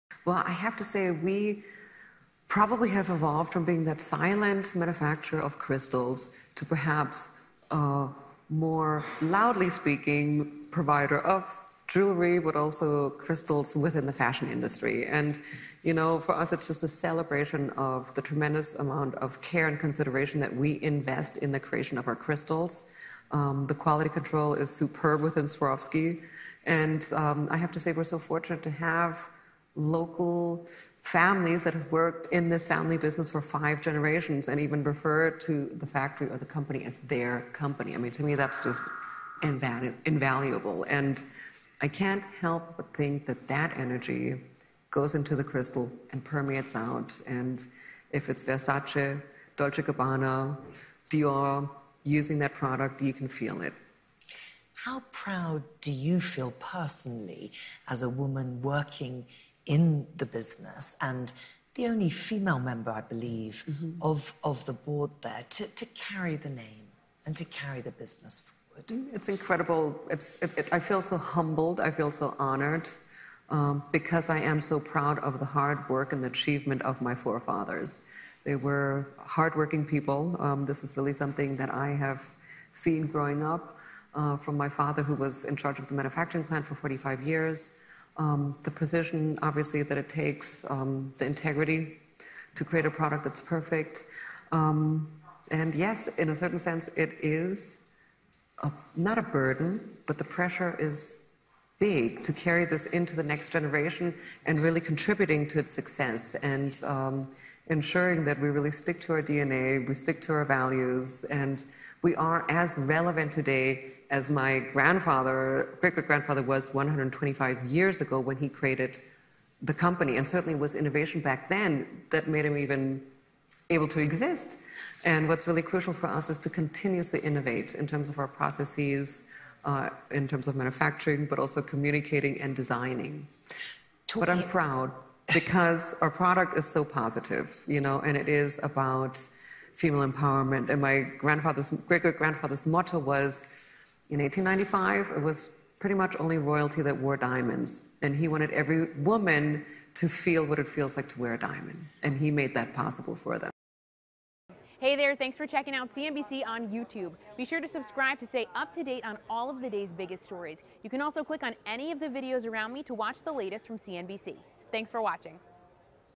Quality control is ‘superb’ within Swarovski, says exec board member | World Economic Forum
Nadja Swarovski talks about the jeweler’s evolution and the creation of its crystals.